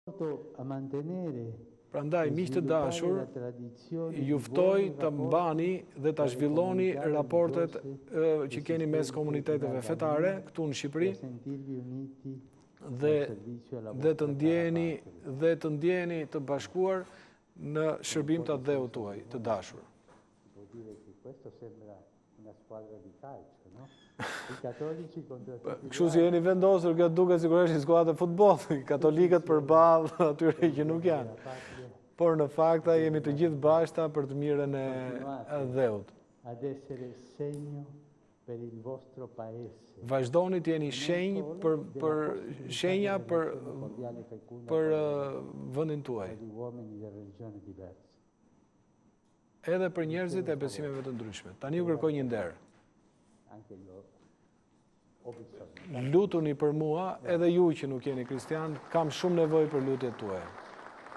Batuta e Papës: Të ulur si dy skuadra futbolli, por bashkë- RTV Ora News- Lajmi i fundit-
Vizita e Papa Françeskut në vendin tonë edhe pse zyrtare nuk ka kaluar pa batuta.

Ati i Shenjtë me thjeshtësinë dhe humorin që e karakterizon teksa gjendej në një takim me komunitetet fetare në Shqipëri në Universitetin “ Zonja e Këshillit të Mirë”, i ka “tërhequr” vëmendjen organizatorit për mënyrën e pozicionimit të të pranishmëve.

"Kështu si jeni vendosur duhet sikur është një skuadër futbolli katolikët përballë atyre që nuk janë, por në fakt jeni të gjithë bashkë për të mirën e atdheut. Vazhdoni të jeni shenja për vendin tuaj”tha Ati i Shenjtë duke e shoqëruar këtë moment me buzëqeshje.